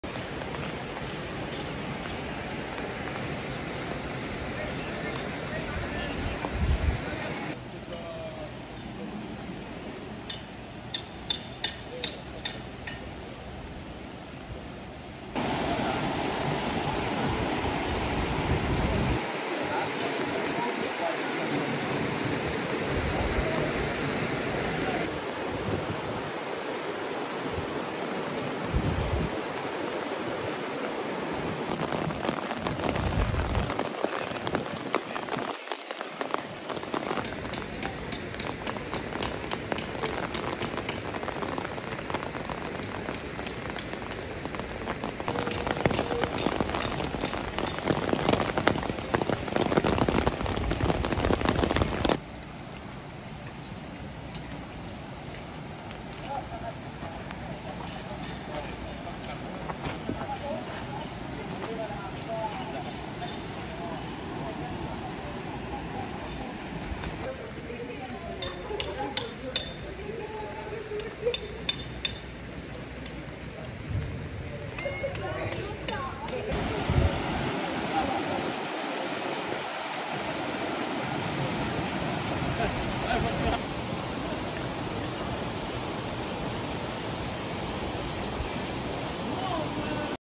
Maltempo Blevio (Co), la frana: auto sommerse dai detriti
Si contano i danni a Blevio, dove una forte ondata di maltempo si è abbattuta nel pomeriggio, causando una frana che si è staccata nella zona di Capovico. Auto e garage distrutti.